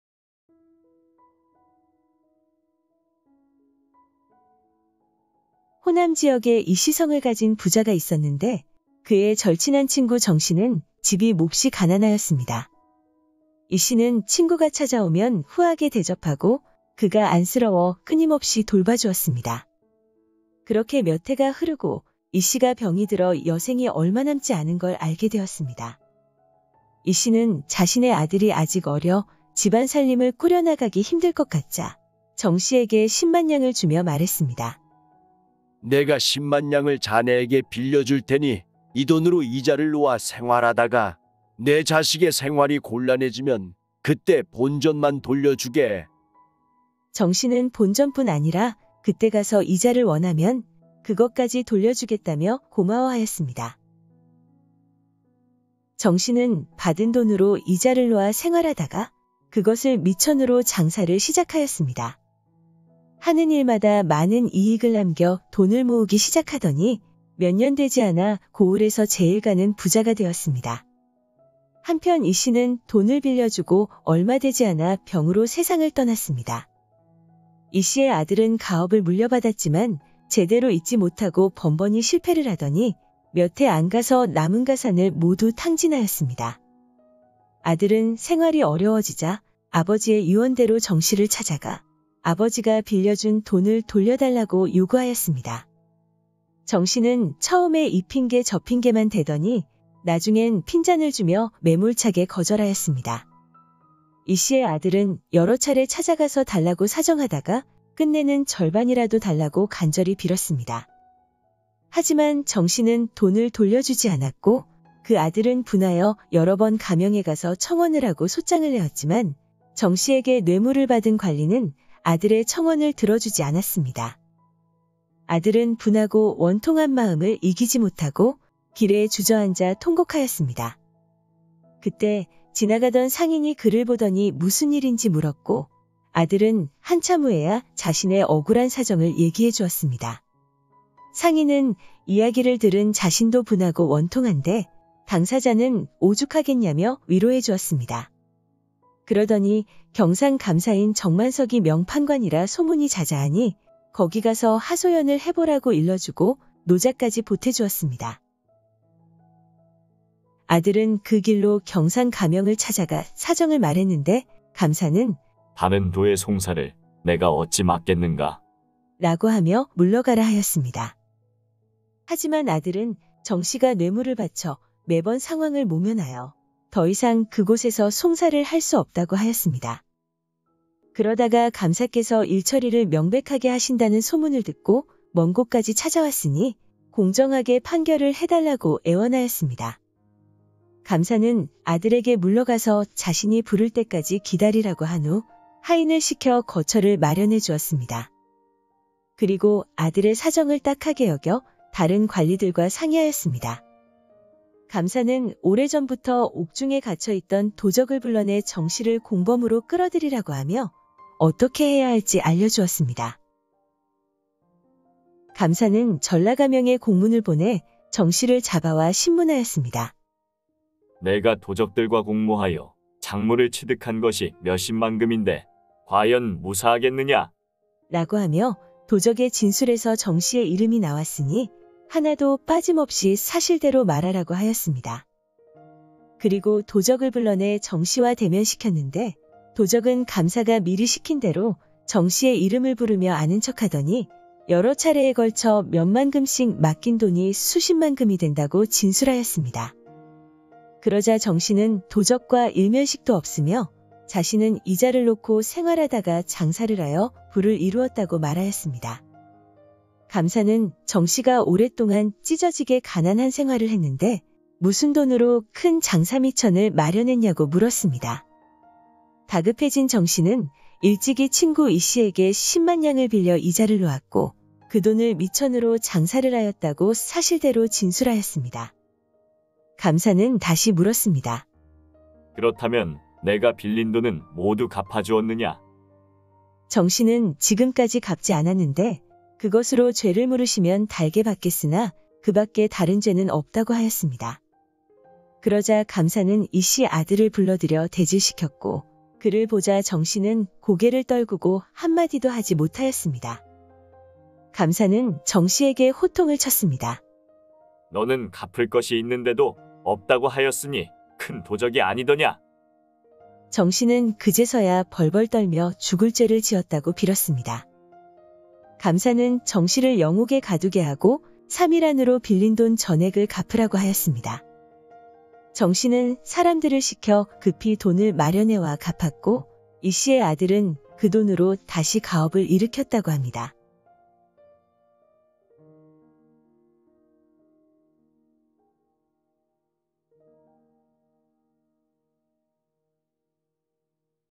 배신자와 명판관 - [정만석]]. 야담. 야사. 민담. 설화. 전설
부자 이씨는 가난한 친구 정씨가 안쓰러워
오랫동안 그를 돌봐주었습니다.
어느날 이씨가 병이 들자 친구에게 10만냥을 빌려주며
그 돈으로 생활하다가 자신의 어린 자식이 힘들어지면
그때 돌려주라고 하였습니다.
이씨가 죽고 아들이 장성하여 정씨에게 빌린 돈을
돌려달라고 하자 정씨는 매몰차게 거절하는데.
[계압만록]과 [만오만필]에 소개된 이야기